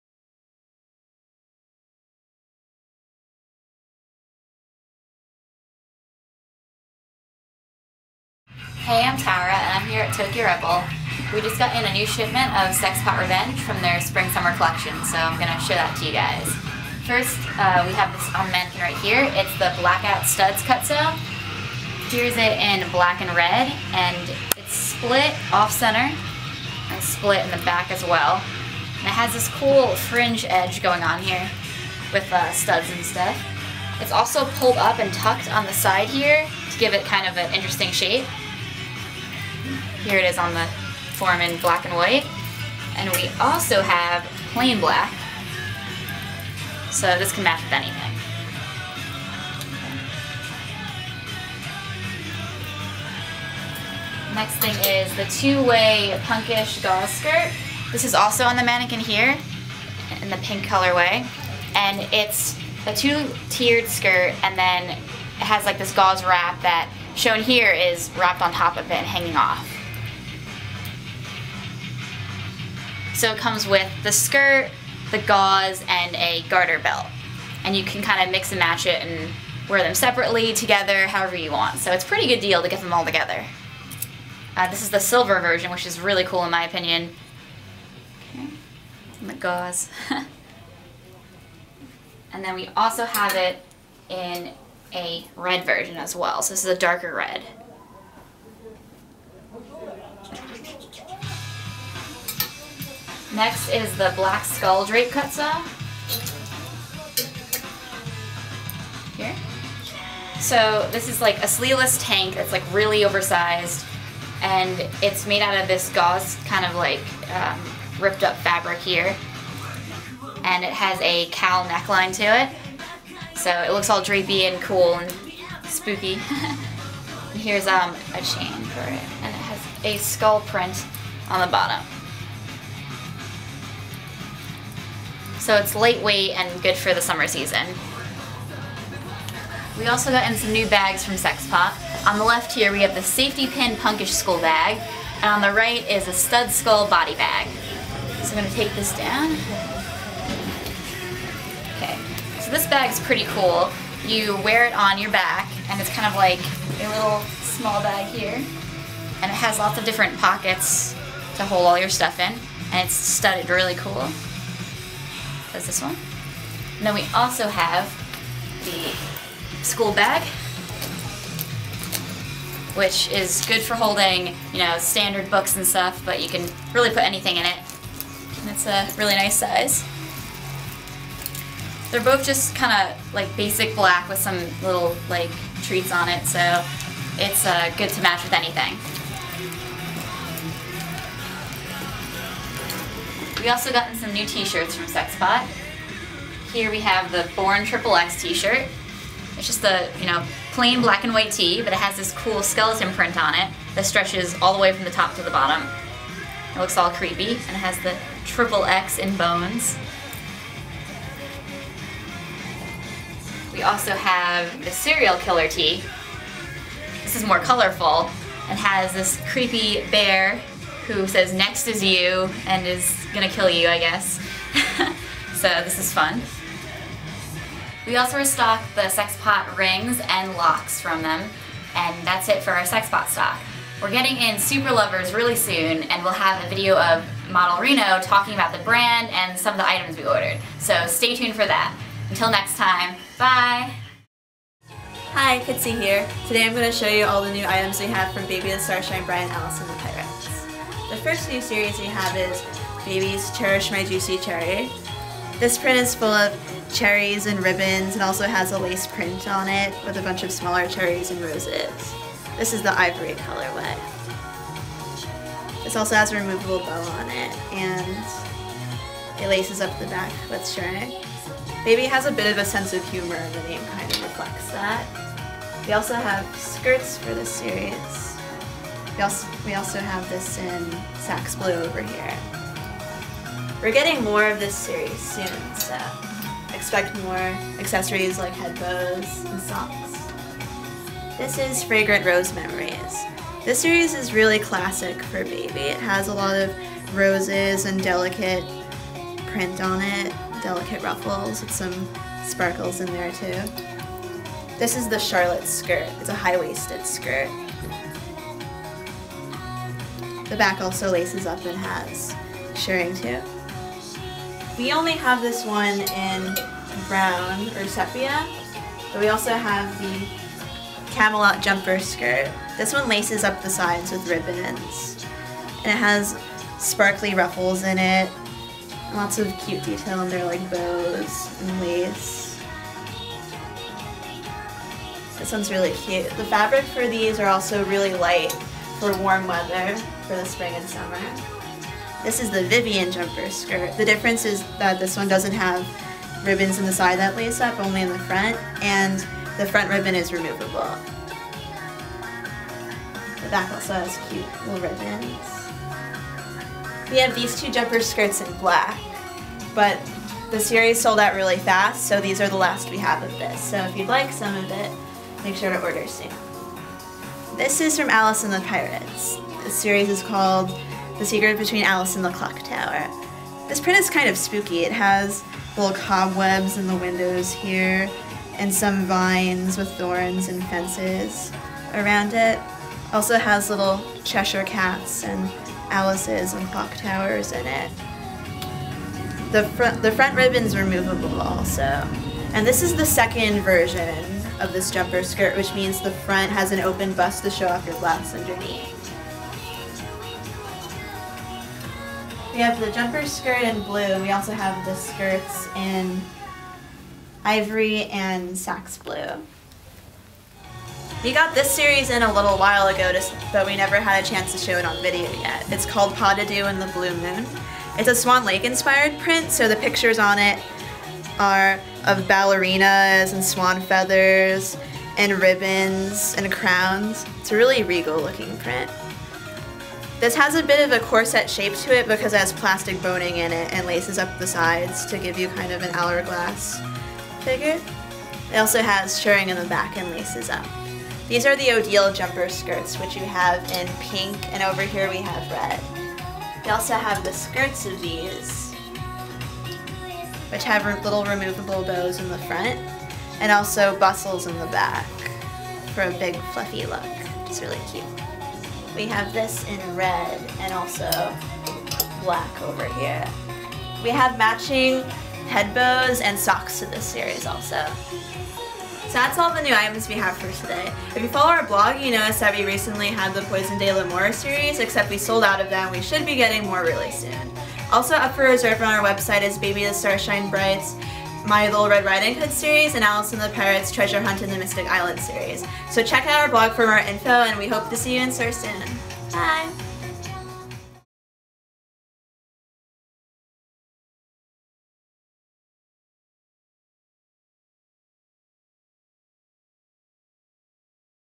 Tokyo Rebel Stock Update vol. 29
It's a special week when we have both Tara and Kitsy in the same stock update video! Here they show you our latest products from SEX POT ReVeNGe and Baby, the Stars Shine Bright/Alice and the Pirates. New cutsews, bags, skirts and more from SPR, while from Baby/A&P we have the Cherish my juicy cherry series, Fragrant Rose Memories, The secret between ALICE and the clock tower, and Pas de deux in the blue moon.